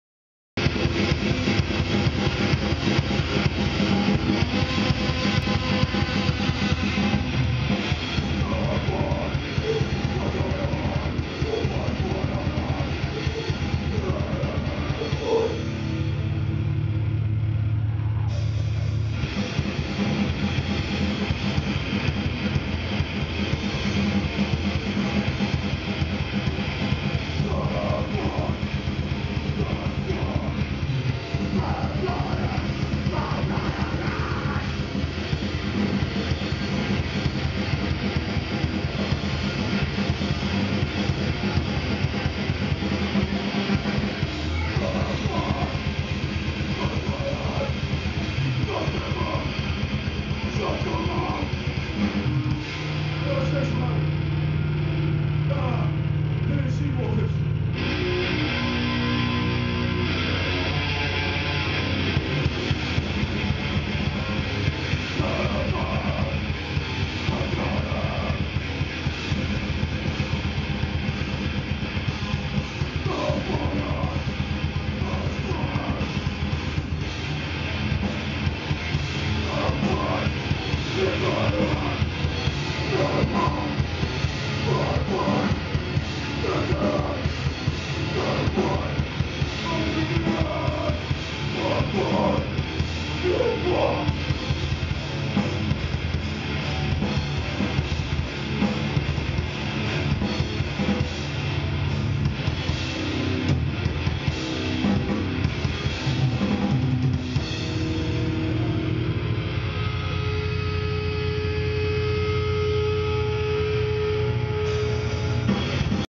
Human Cull @ Plymouth White Rabbit supporting Wormrot 2012
UK Grind trio Human Cull (formerly Gran Toucher) smashing shit up at their recent gig in Plymouth... Make sure you check these guys out!